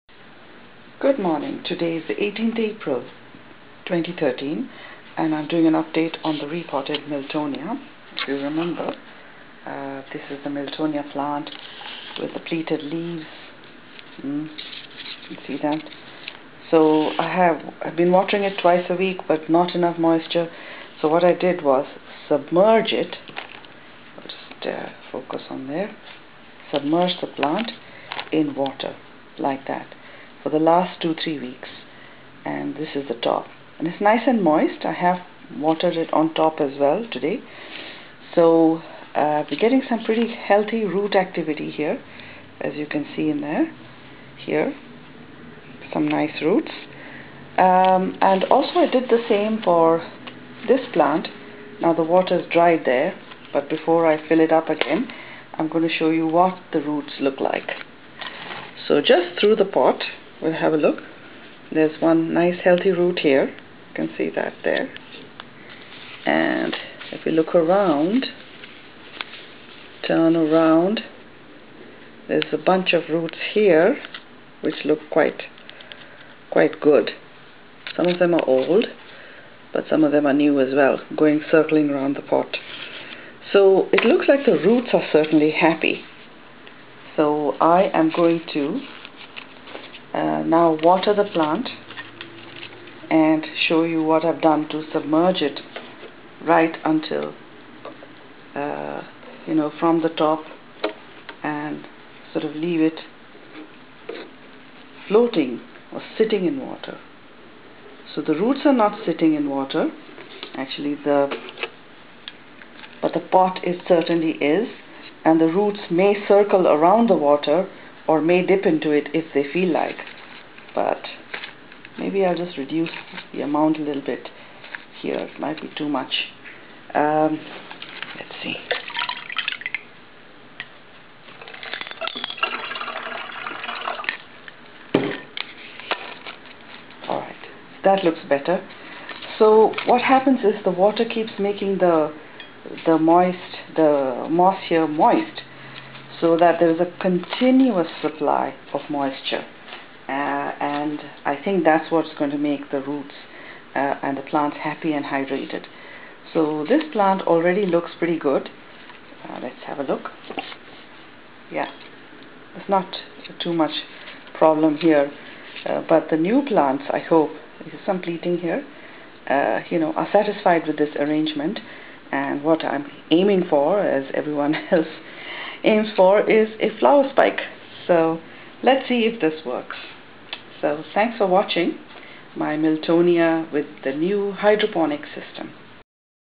Experiments with HYDROPONICS for MILTONIA/ or MILTONIOPSIS.
Hi Viewers. I saw a demonstration at our Orchid society meeting on Hydroponic culture of some orchids. These were potted in Leca ( clay pellets) and the lower third was submerged in water to supply continous moisture to the medium. The roots grew right down just skimmimg the water surface ( not dipping into it, and looked pretty healthy. I am trying this approach for the Miltonia which needs continous moisture. I had in fact been away the last three weeks , and left the plants submerged like this, and the plants did well for three weeks in my absence. If this works then I plan to use this for all my orchids when I am away!. Has anyone ever tried this themselves?